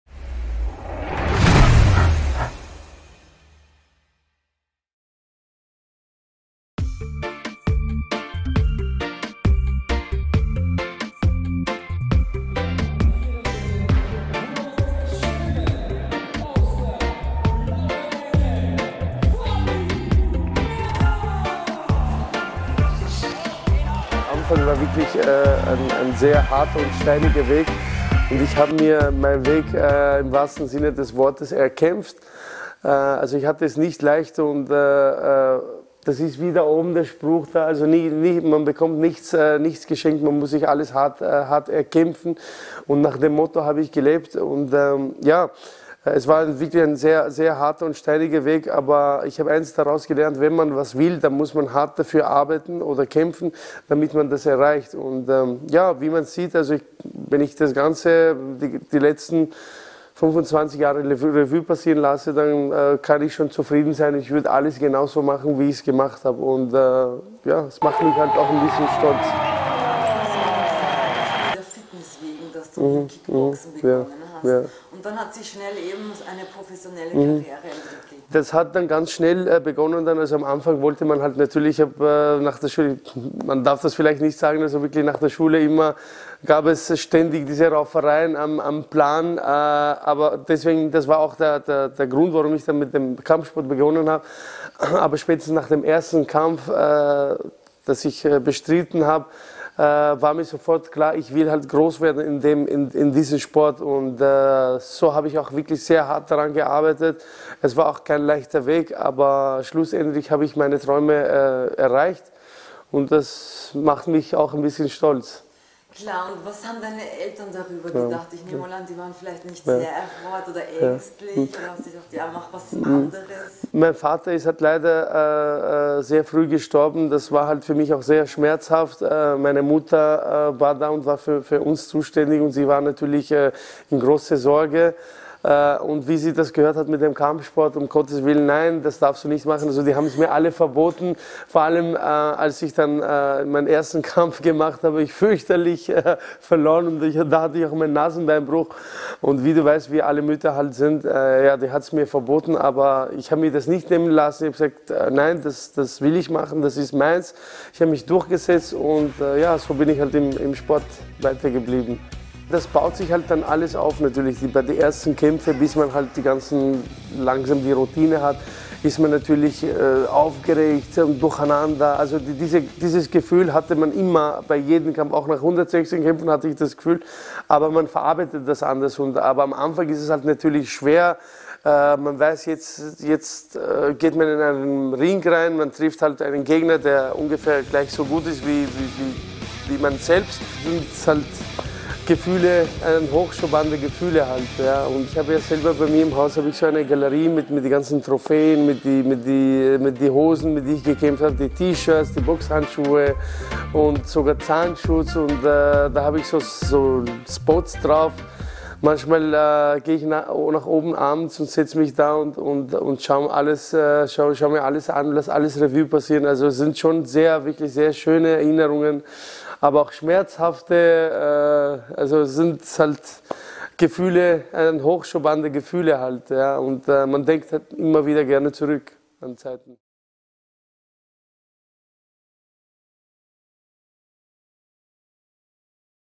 Fadi Merza im Interview mit KS1-TV
Fadi Merza, sechsfacher Weltmeister im Thaiboxen, kam einst als Einwanderkind mit 10 Jahren nach Wien. Hier begann er mit dem Kampfsport - anfangs um sich im Schulhof behaupten zu können, dann wurde daraus eine ernstzunehmende Kampfsportkarriere im Kickboxen dann im Thaiboxen. Heute blickt er stolz auf viele Jahre Profikarriere zurück. Zwar genießt der gebürtige Syrer seine sportliche Pension, doch faul herumliegen ist nicht sein Ding. Seit 2017 ist er glücklicher Sportstättenbesitzer der "Team Merza Academy". Wir trafen ihn dort und plauderten etwas über seine Karriere als bekanntester Thaiboxkämpfer Österreichs.